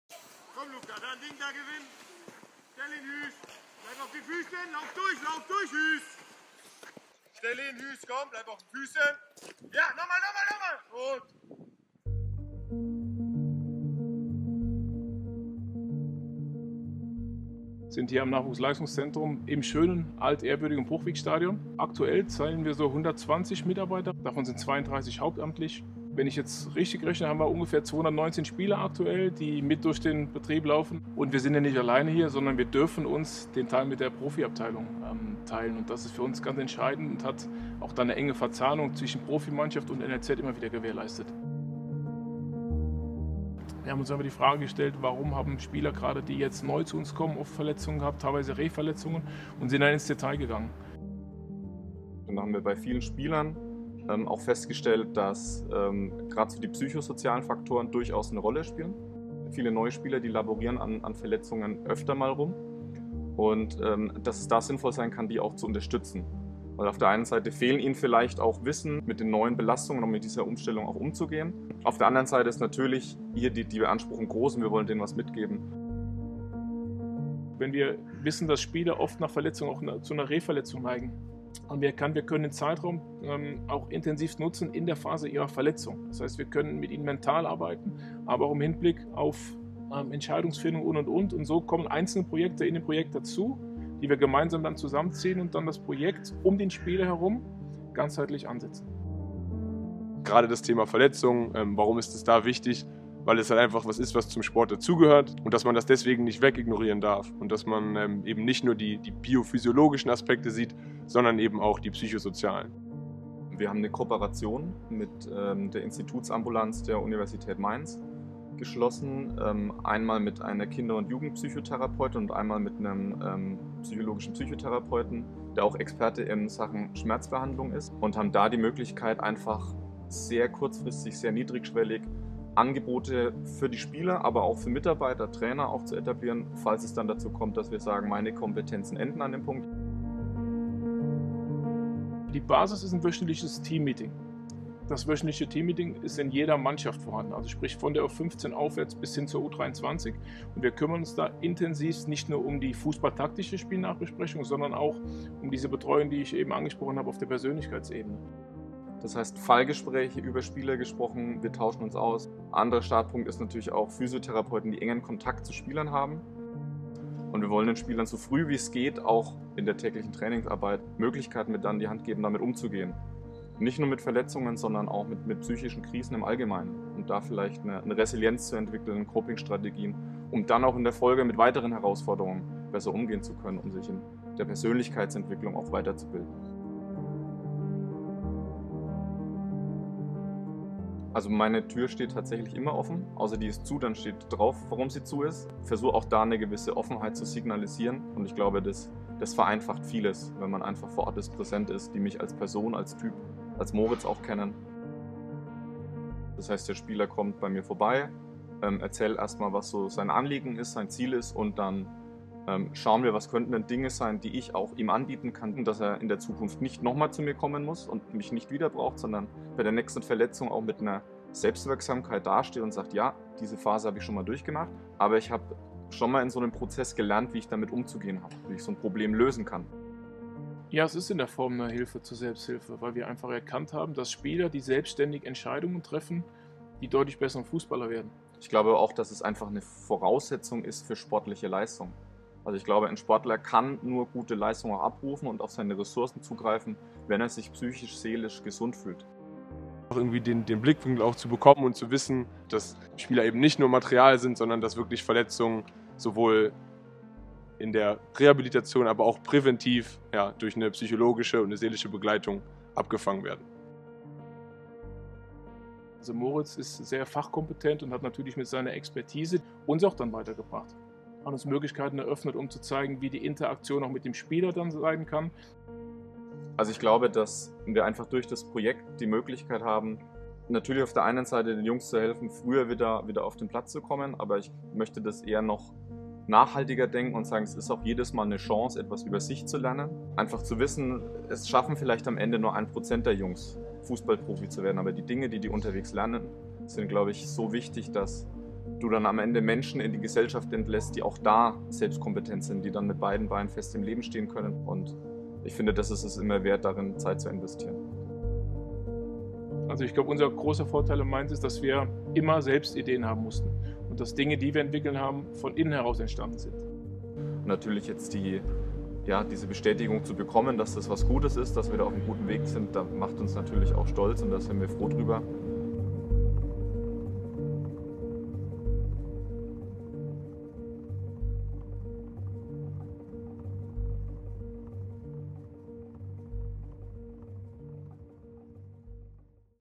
Mainz 05 erhält Förderpreis der Robert-Enke-Stiftung | Saison 2021/22
Der 1. FSV Mainz 05 ist von der Robert-Enke-Stiftung mit dem Förderpreis 'Seelische Gesundheit im Nachwuchssport' ausgezeichnet worden. Im Rahmen einer Preisverleihung am zwölften Todestag von Robert Enke überreichten Teresa Enke, Vorstandsvorsitzende der Stiftung, Oliver Bierhoff, Direktor Nationalmannschaften und Akademie des DFB und Schirmherr Mats Hummels den mit 10.000 Euro dotierten Preis für das Projekt 'Psychosoziales Verletzungsmanagement', den NLZ-Sportpsychologe Moritz Hirmke, Junioren-Cheftrainer Jan Siewert und Constantin Kuhlmann stellvertretend für das Nachwuchsleistungszentrum der 05ER entgegennahmen.